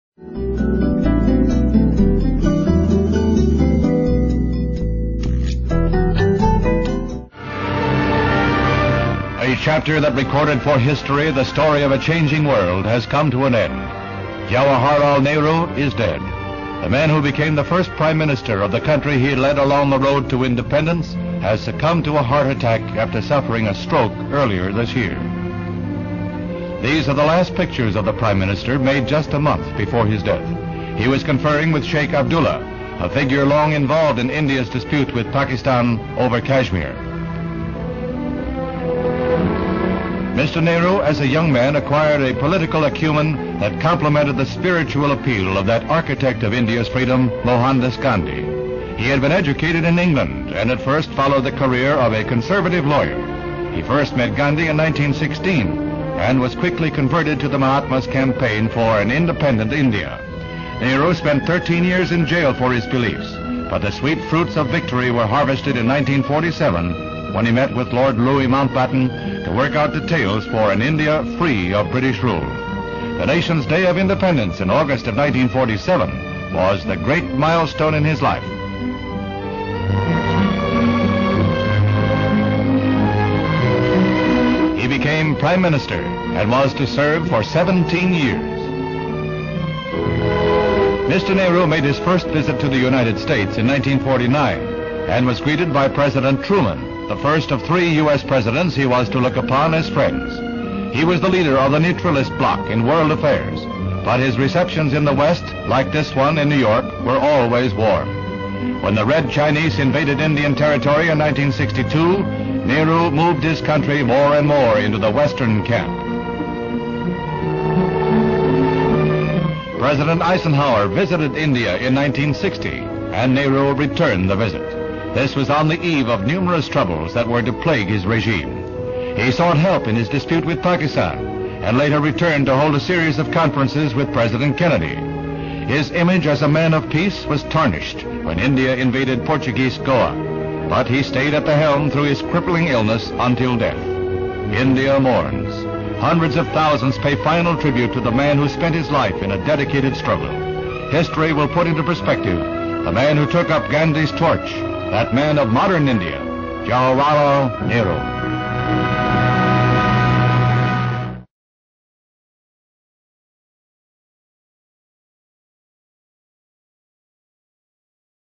Man Of Peace,Man Of Power,Man Of War (USA 1949)-Jawahar Lal Nehru ! The turbulent control-1960s.
Battle with Organized Crime and John.F.Kennedy's Support.
WATCH THE VIDEO CLOSELY TO SEE WHAT HAPPENED TO NEHRUJI !